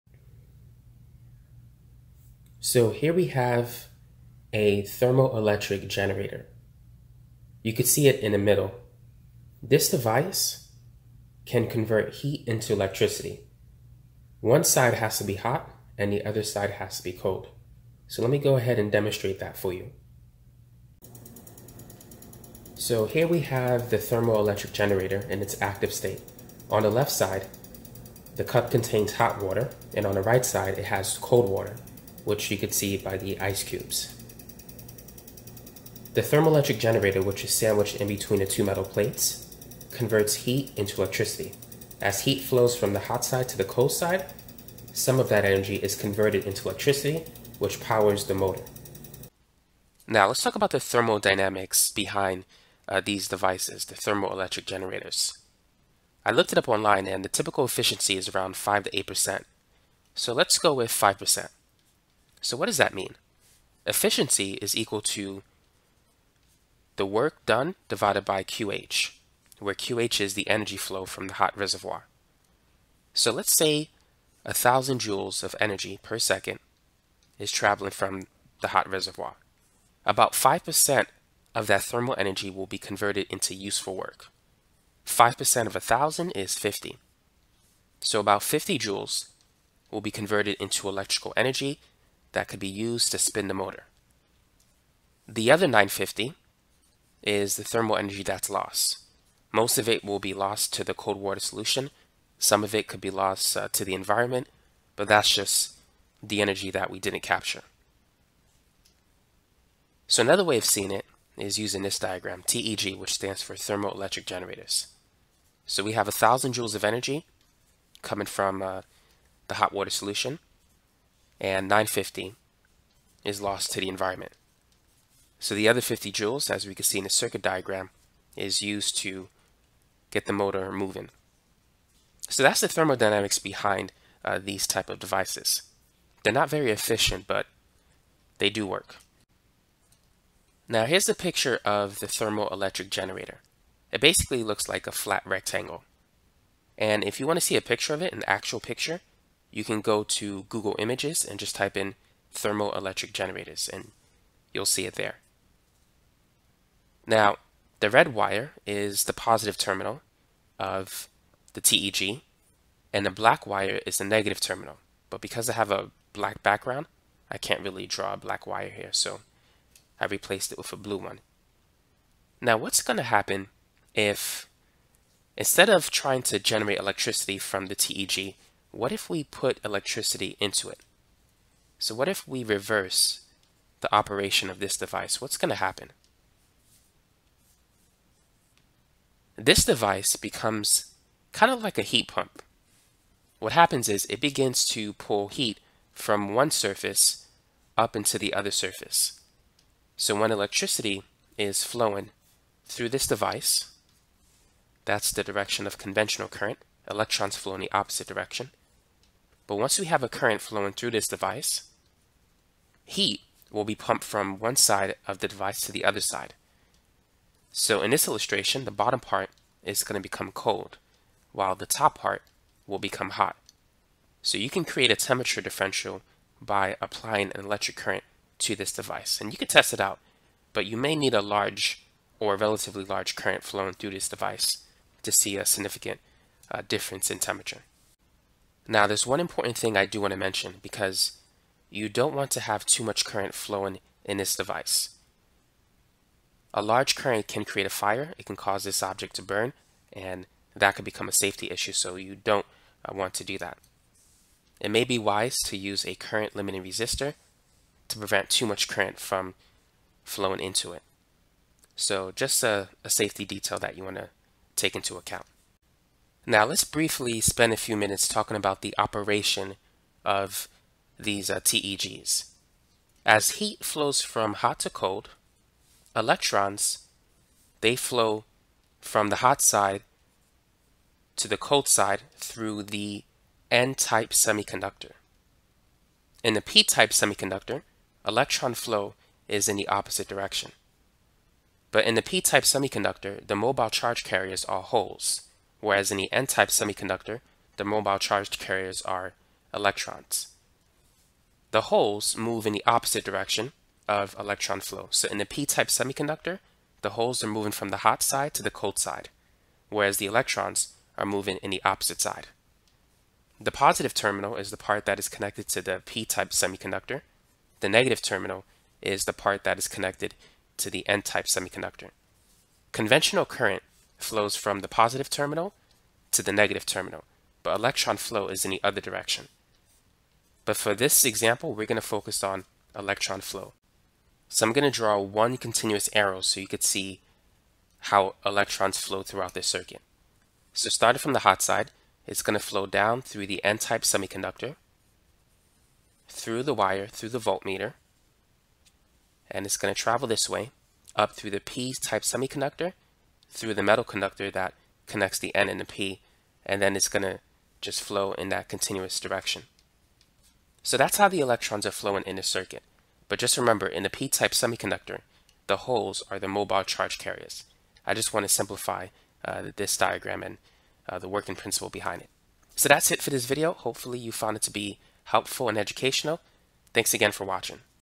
Thermodynamics - Converting Heat Energy Into Electricity Using a Thermoelectric Generator
This thermodynamics video tutorial explains how to convert heat into electricity using a thermoelectric generator.  This device takes advantage of a temperature differential between its two sides converting the heat flow into an electric current.  The TEG is composed on n-type and p-type semiconductors which forms a thermocouple.  Two effects that are related to the use of this device are the seebeck effect and the peltier effect.

Peltier Thermoelectric Cooler

Amazon Store